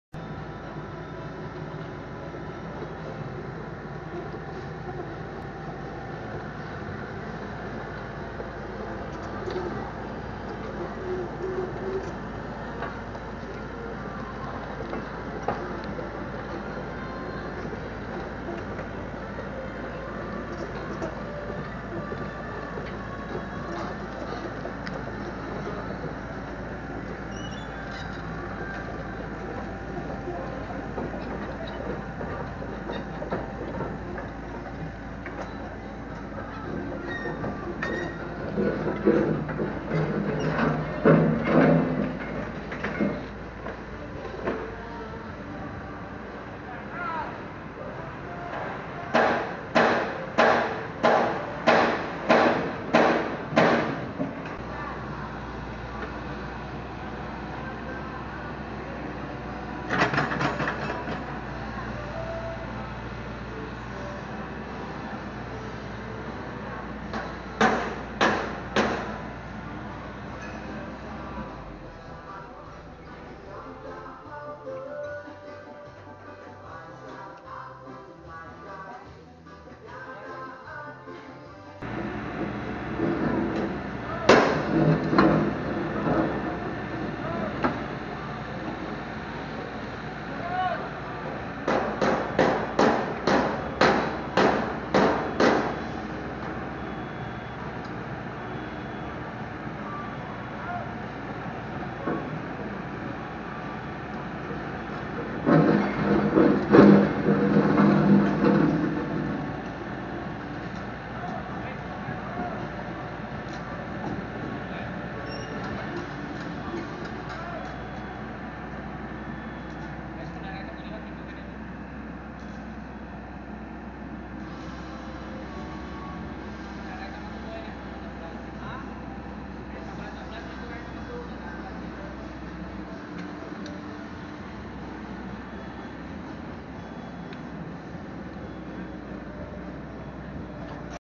Mini Excavator Komatsu PC78 Pulling Kobelco SK200-8 #2
Mini excavator komatsu pc78 sedang menarik excavator kobelco sk200-8 yang alami kerusakan